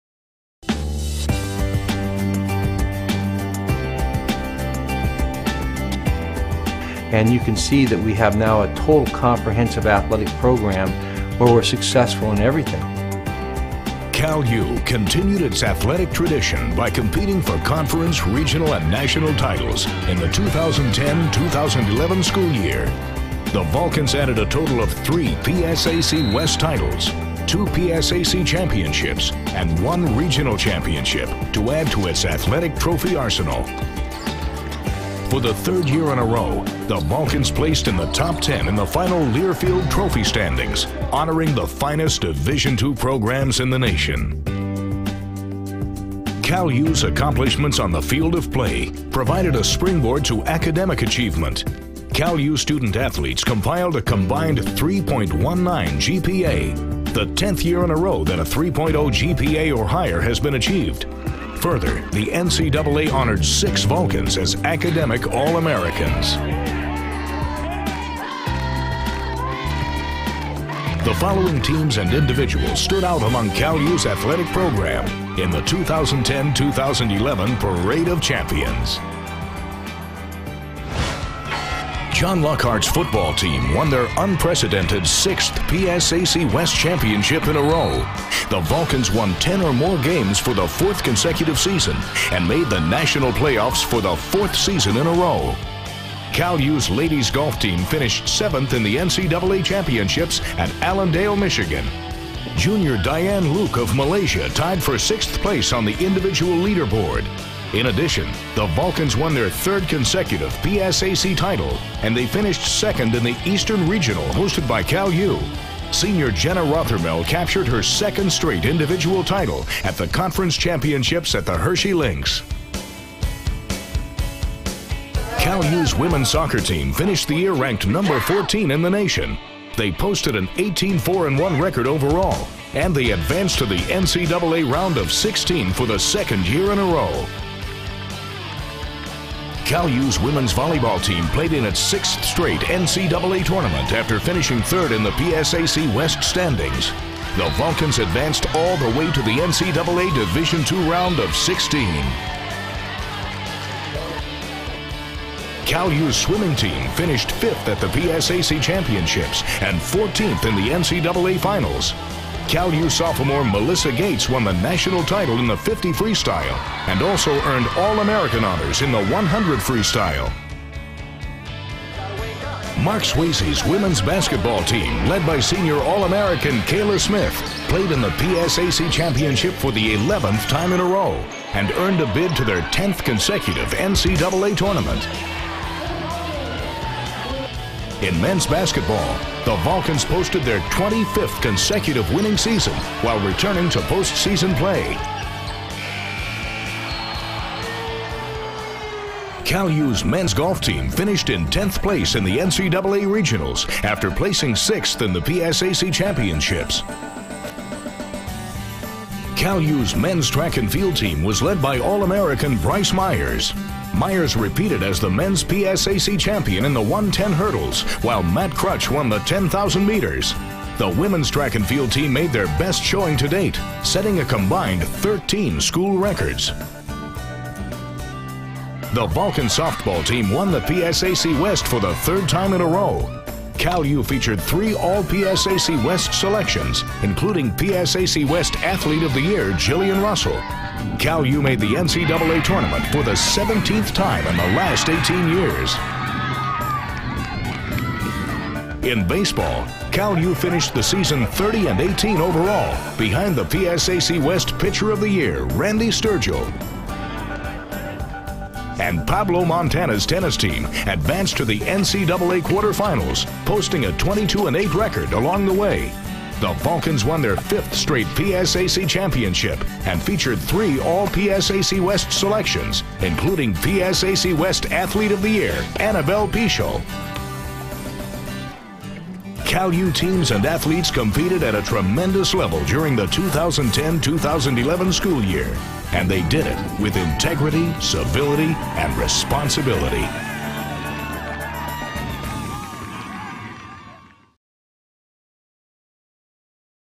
Cal U Athletics in 2010-11
Overview of Cal U Athletics during the 2010-11 academic year, which featured PSAC Championships in women's golf and women's tennis and a National Champion in swimming.